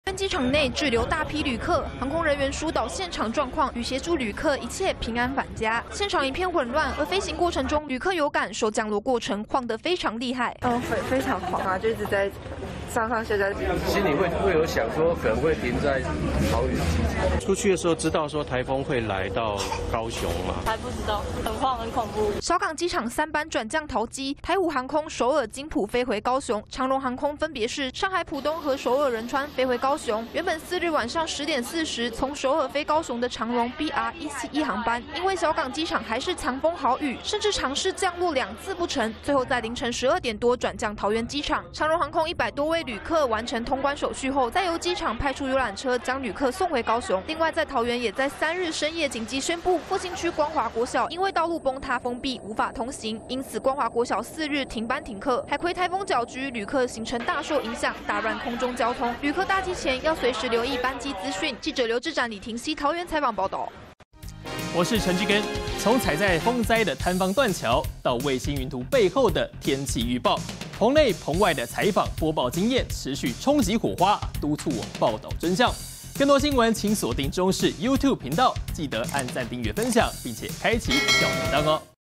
海葵狂風! 飛機降落"上下晃" 旅客嚇:在坐大怒神│中視新聞 20230904
受到颱風影響，桃園光華國小唯一聯外道路崩塌，3日深夜緊急宣布停班停課，而桃園機場則有航班延誤，多架班機起降不順，最後轉降桃機，不過降落過程非常驚險，就有旅客表示"像在坐大怒神"一樣。